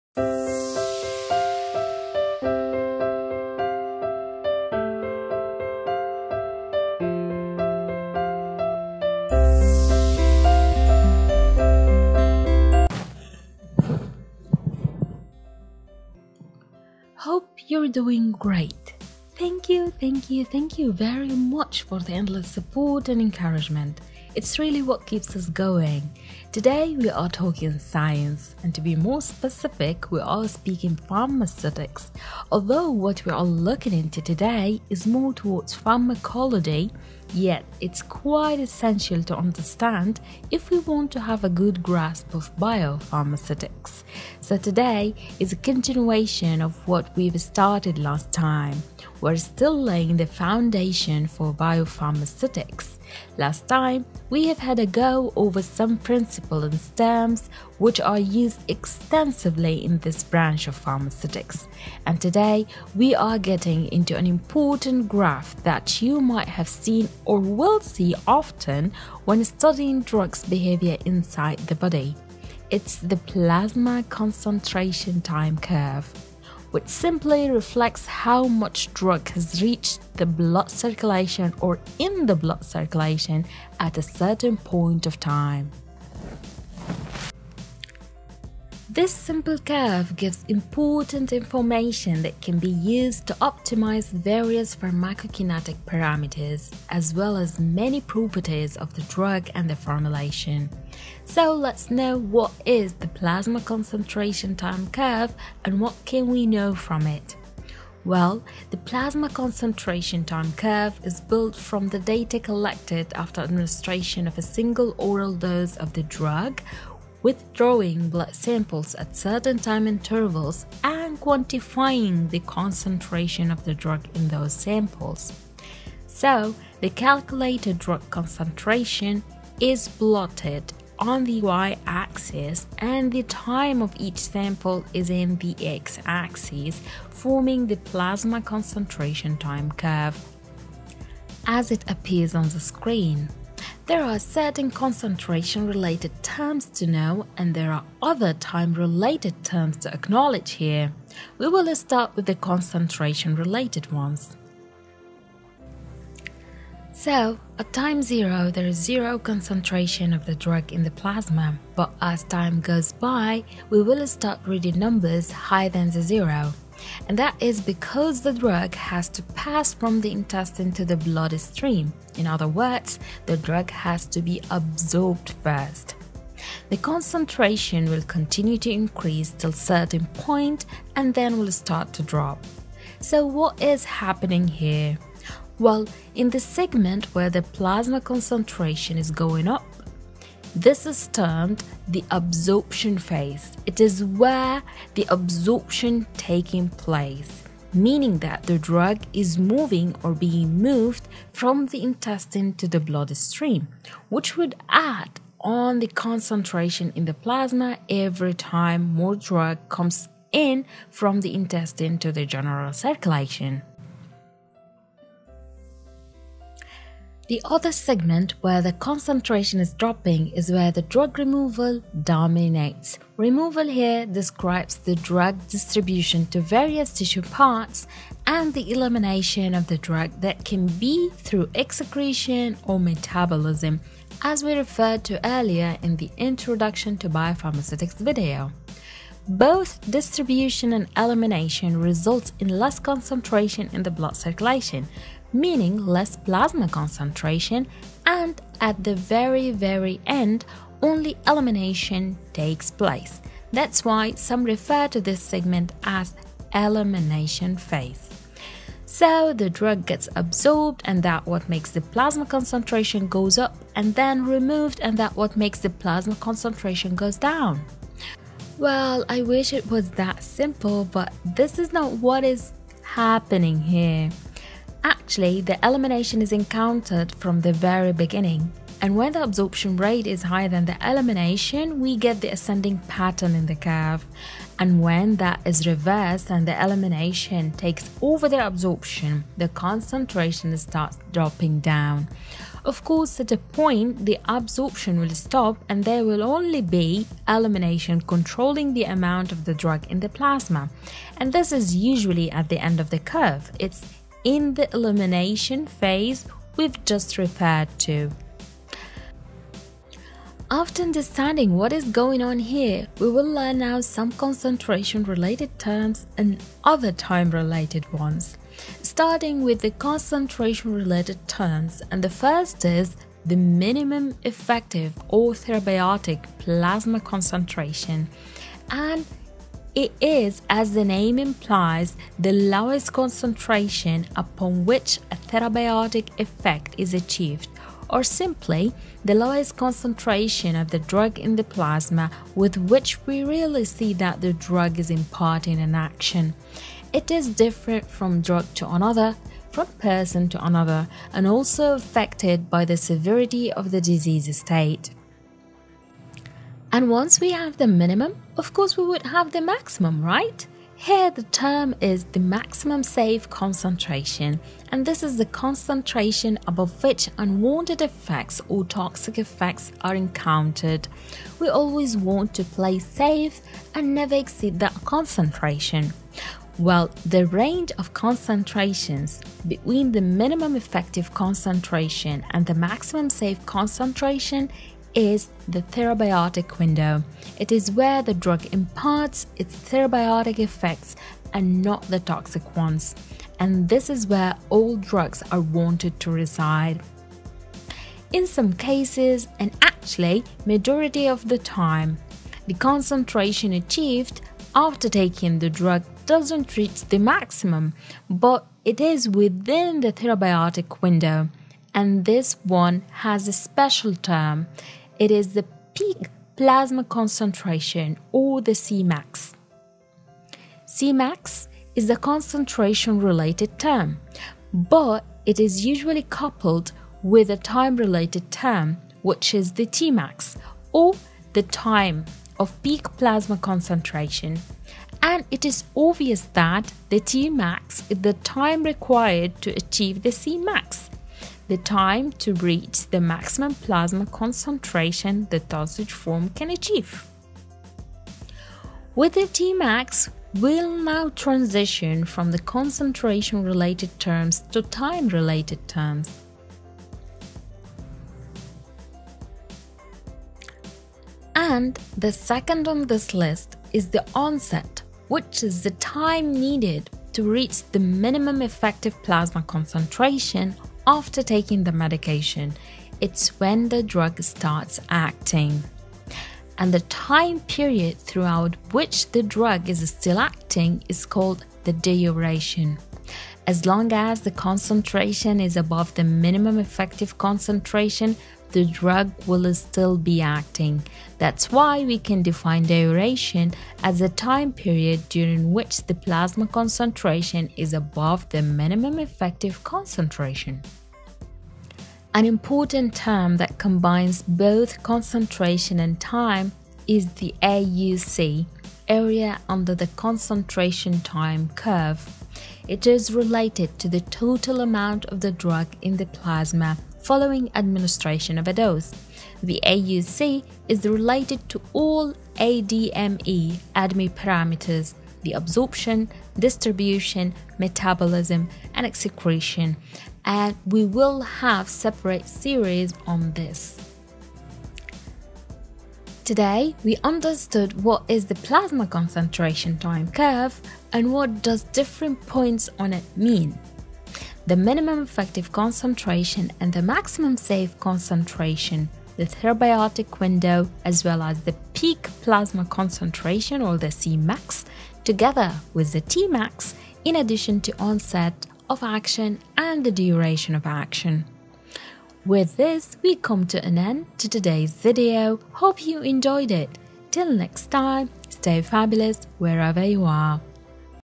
Biopharmaceutics 2 | Understanding the Plasma Concentration-Time Curve & AUC Explained
Hello! 😊
Hope you're doing great! 😍
Today, we're diving into an important concept in biopharmaceutics – the Plasma Concentration-Time Curve! This graph is essential for understanding how drugs behave in the body over time. In this video, we will break down how the curve is constructed and explain key terms such as AUC (Area Under the Curve), Cmax, Tmax, and more. Whether you're new to the topic or reviewing for exams, this video will help you grasp the basics and beyond.